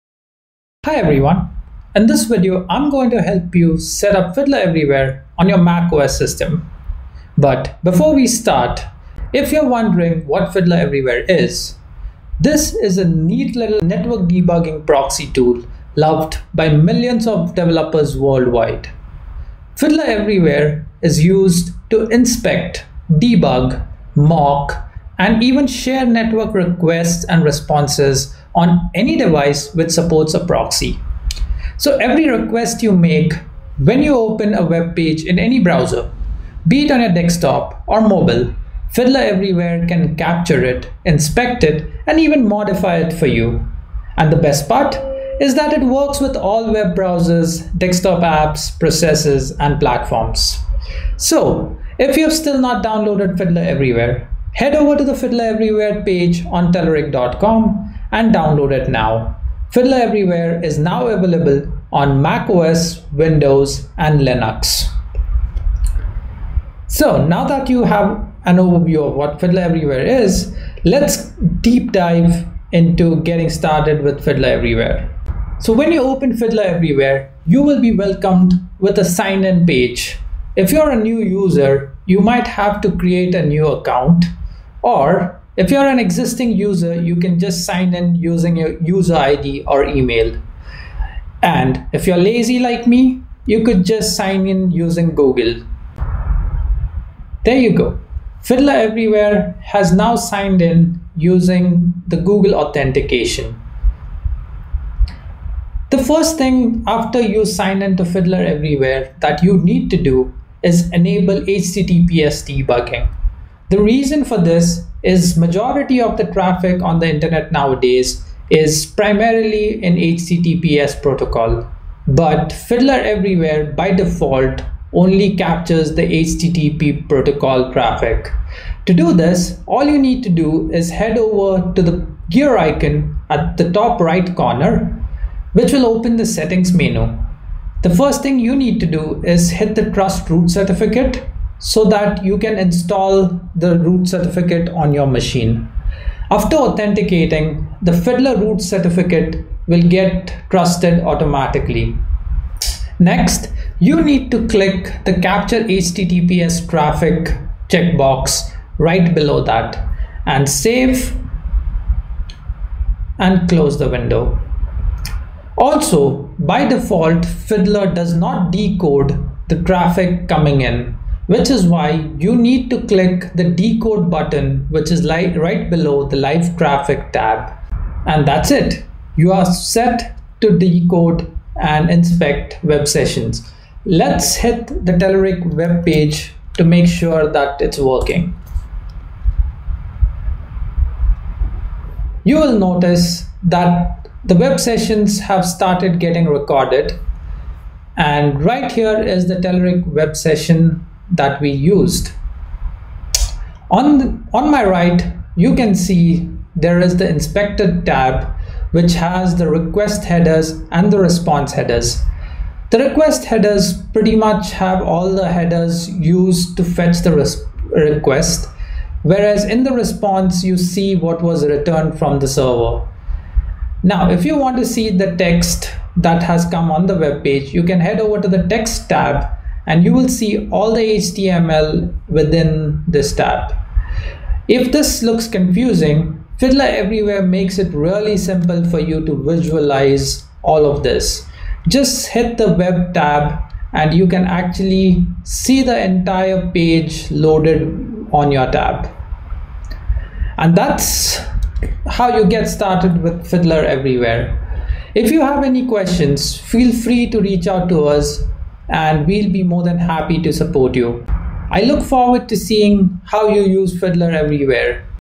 Fiddler Everywhere Quick Start for macOS
The next generation of Fiddler tooling is here! Fiddler Everywhere brings the best of Fiddler Classic forward, improves what you love, and adds built-in team collaboration. Learn how to get started quickly on macOS with this video and download Fiddler Everywhere when you’re ready